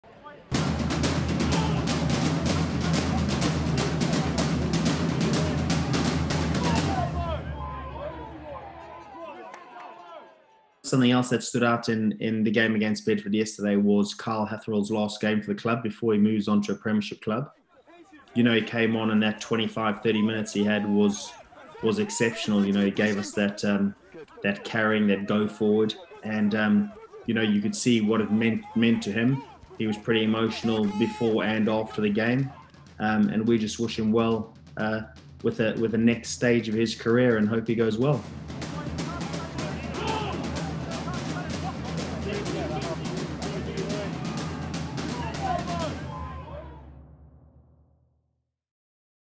Harvey Biljon on Kyle Hatherell's last game for the Reds as he leaves for Worcester Warriors
Second row/back row forward Kyle Hatherell played his last game for the Reds at the weekend in the win at Bedford. Kyle has been signed by Gallagher Premiership side Worcester Warriors. South Africa-born Hatherell, 25 joined the Reds form Scottish Premiership side Marr in 2018, having previously played for Varsity College and Sharks Under-21s in South Africa.